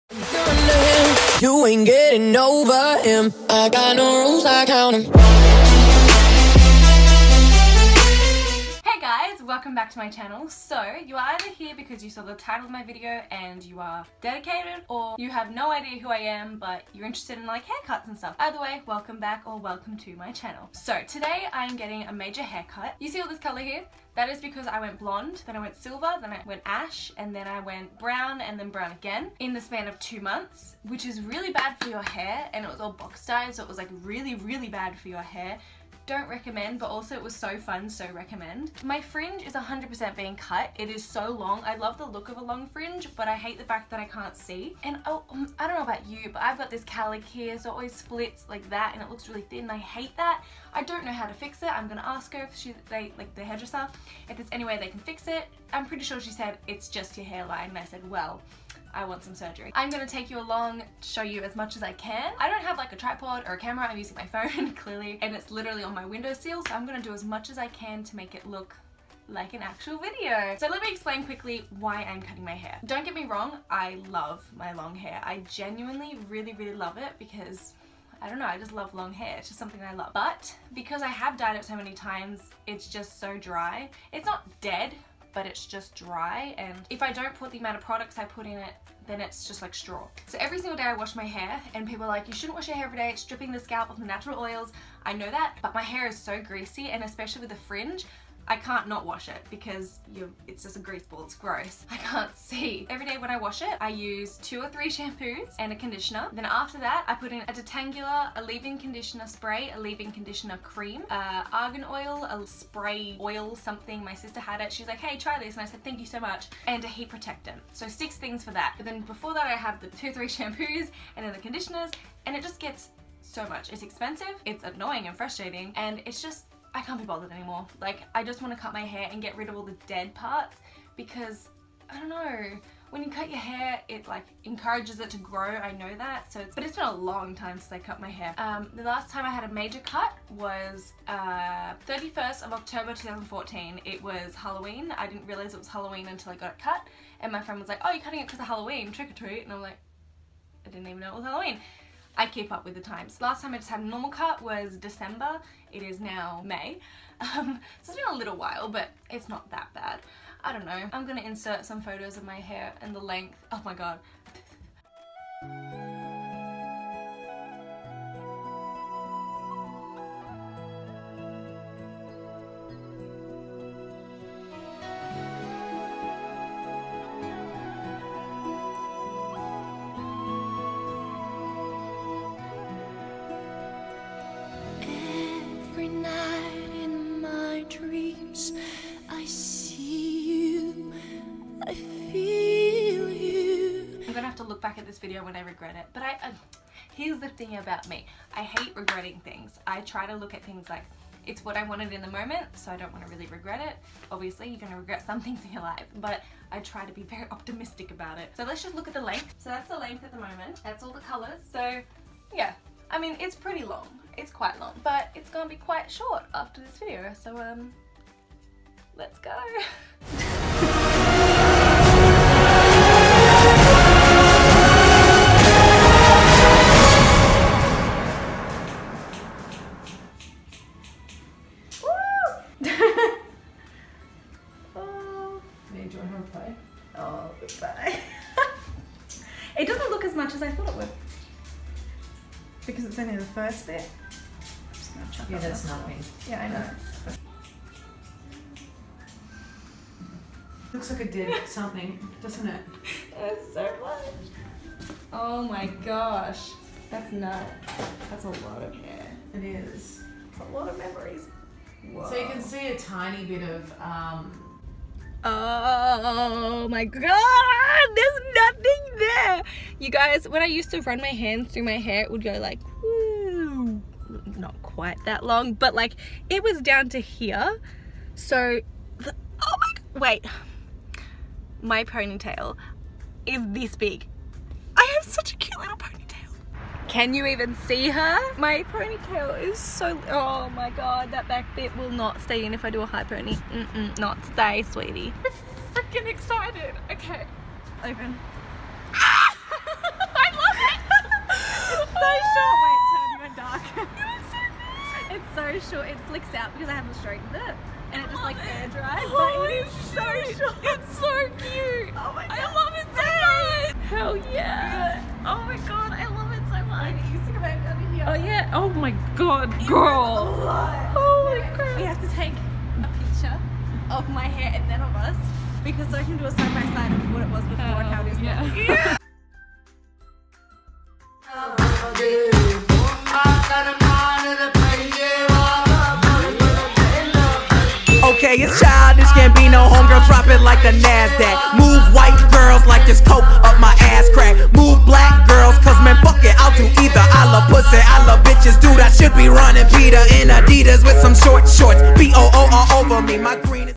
CUTTING MY HAIR SHORT + REACTION FROM BFF!
GUYS I finally cut my hair! YAYYYYY making this video made me love my long hair so much but at the same time I was SO over dealing with it and this change was much needed! I wanted to cut my hair short for about a year now but never did it because I wanted my hair long for my Grade 12 Formal (which was the end of last year). So glad I did this, stilll kinda shook though ahha comment your thoughts!

Personal - Snapchat (kaylasrose)